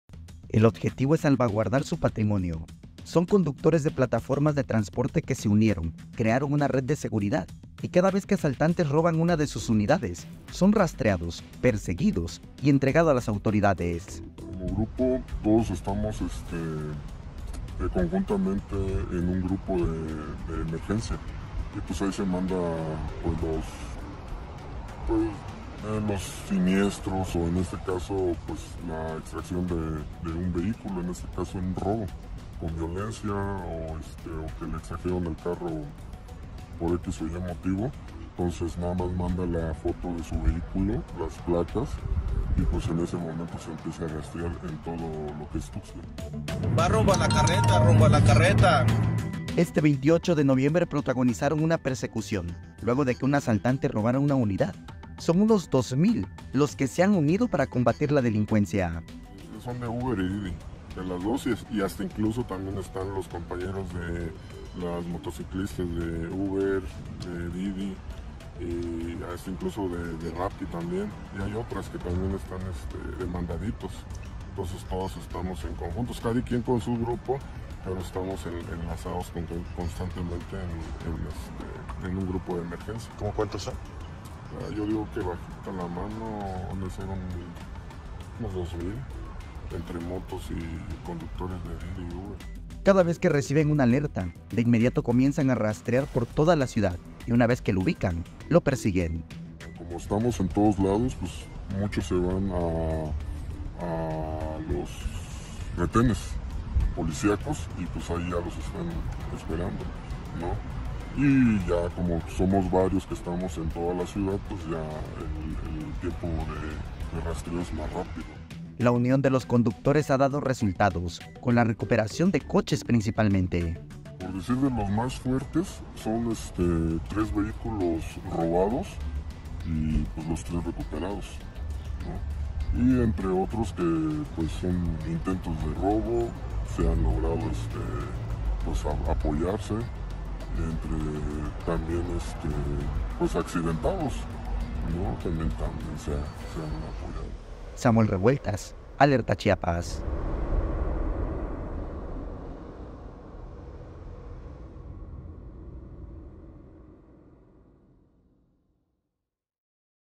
Conductores se revelan ante la inseguridad
Son unos 2 mil los conductores de Uber, Didi, Rapid, Uber eats y Mandaditos que están enfrentando a los delincuentes, al crear una red de seguridad.